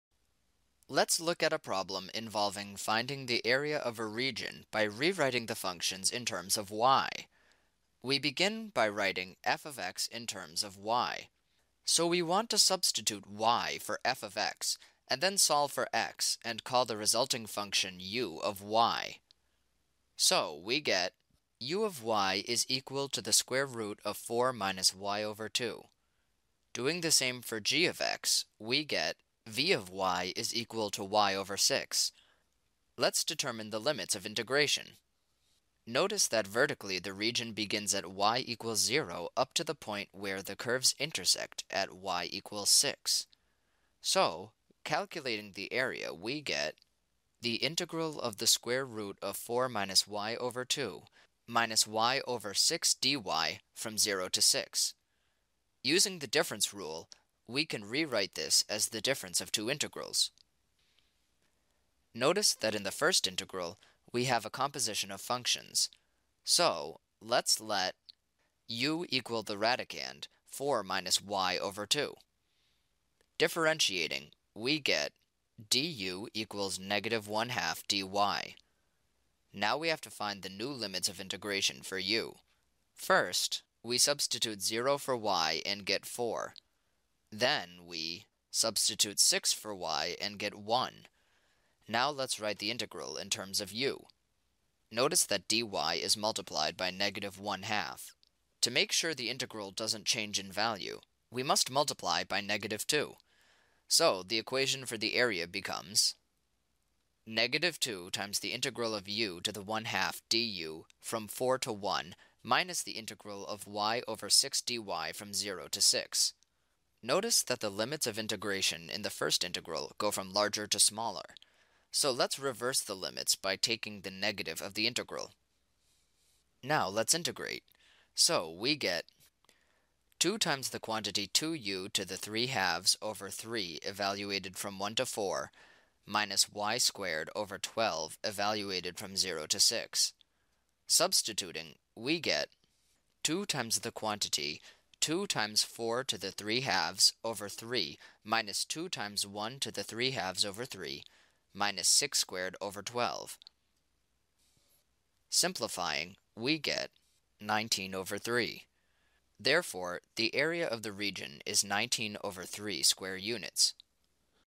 Knewton IV LO 18
Find the area between two curves by rewriting functions as a function of y